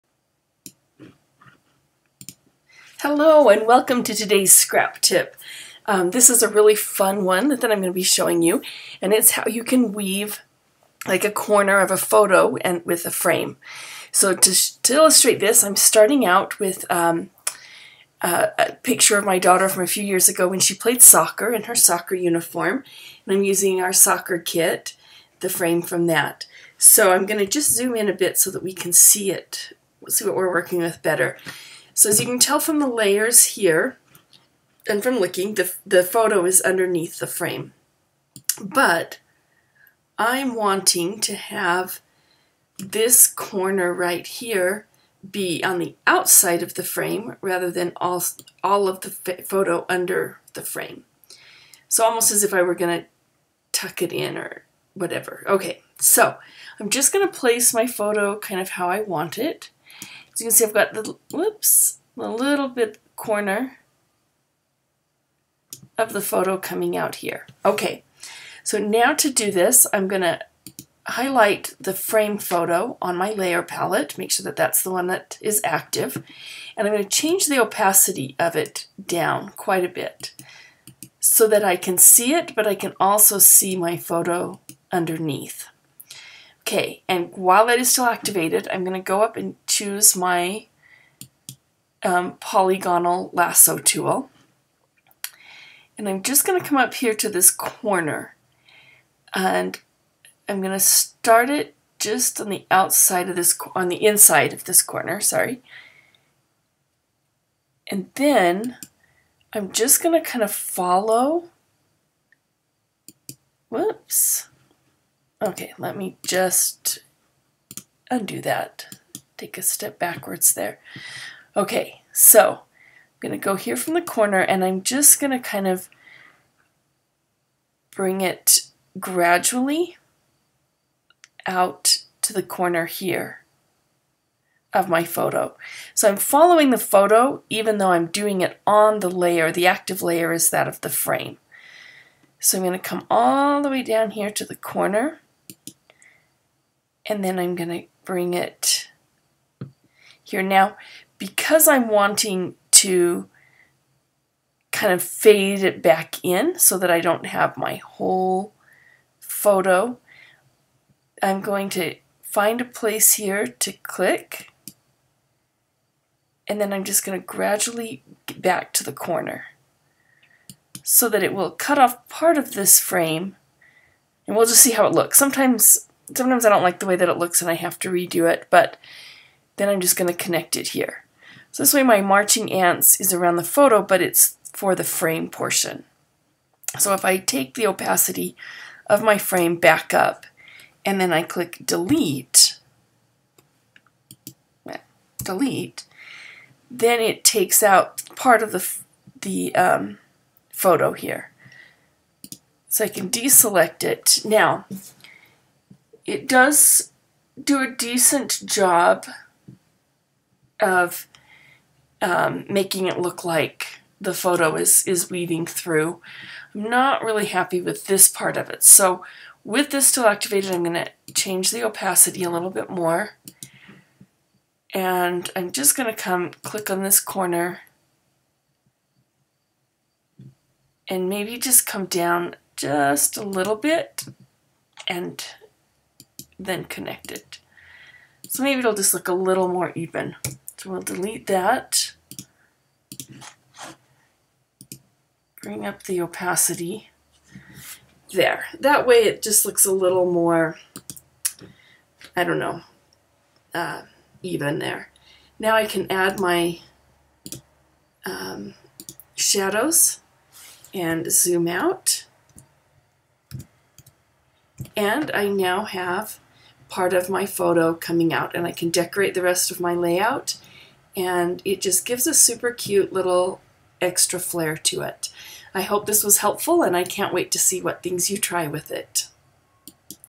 How to Weave a digital photo with a digital frame
A quick tutorial to show you how to weave a photo corner with a frame on a digital layout. Easy to do and gives a little extra flair to your layout! For more tutorials or classes on Digital Scrapbooking, check out my